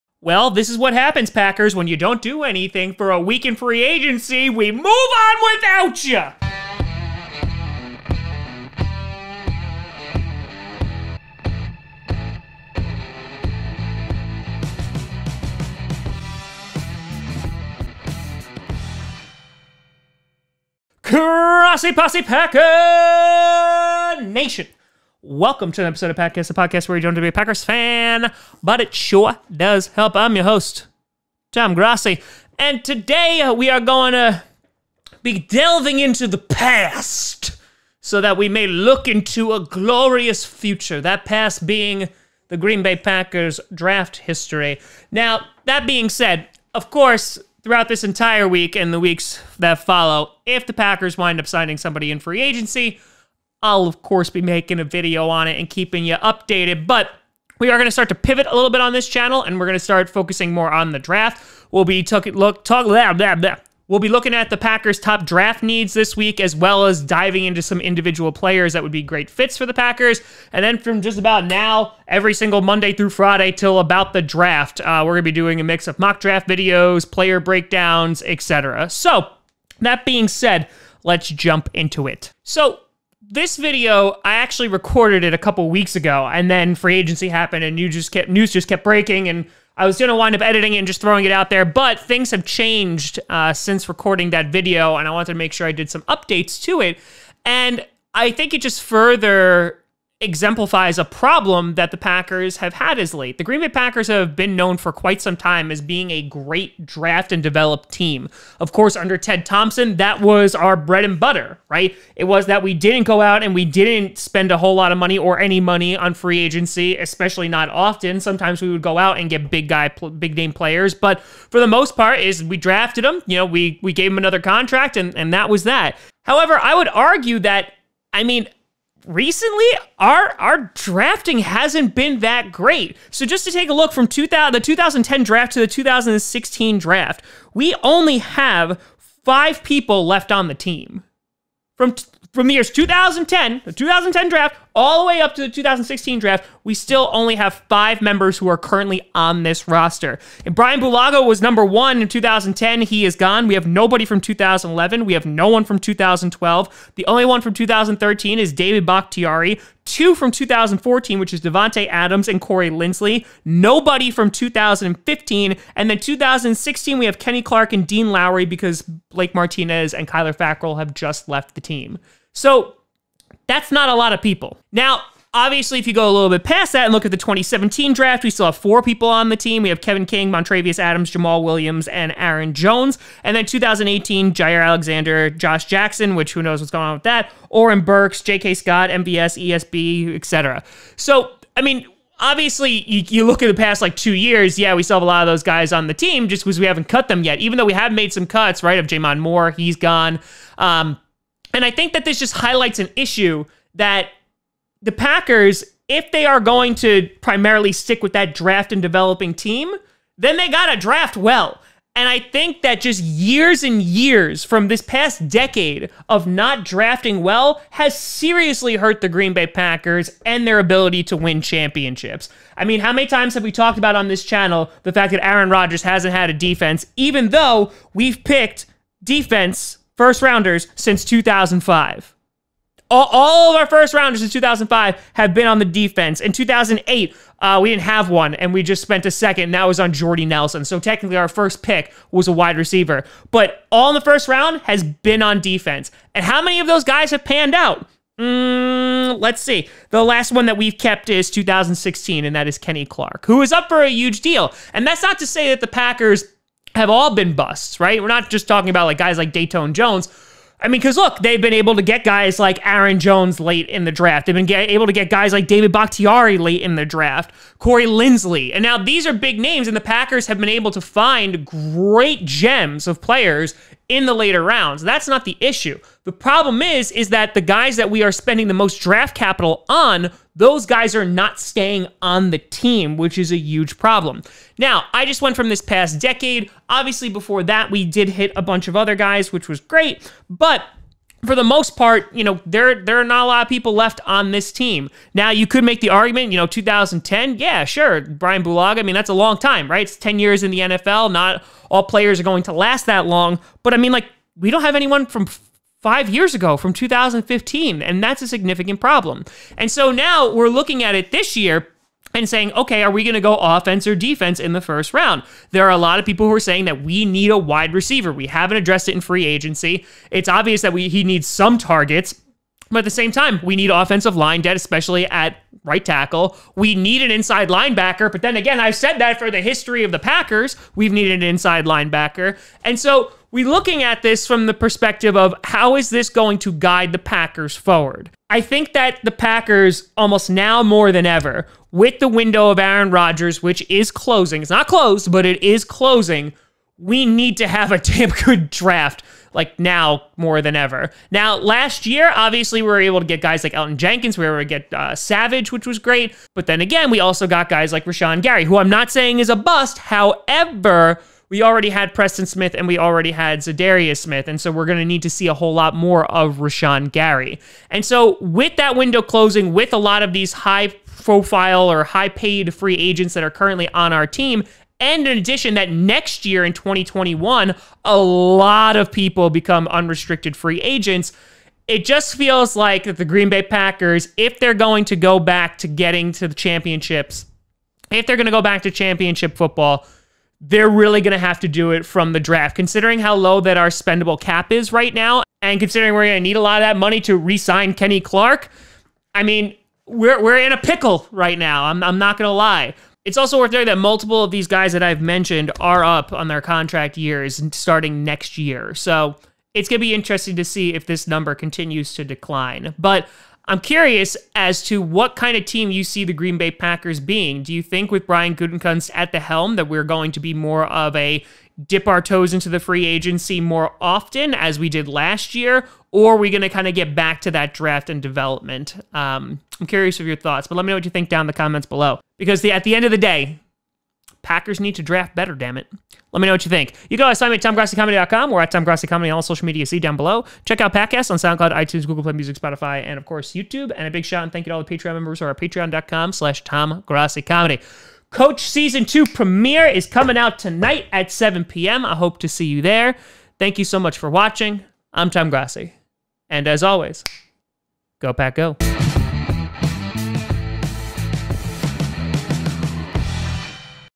The Packers Need to Draft Better
The Green Bay Packers have the reputation of being one of the best teams in the draft each year. If the Packers are to return to the Super Bowl, they need to draft better.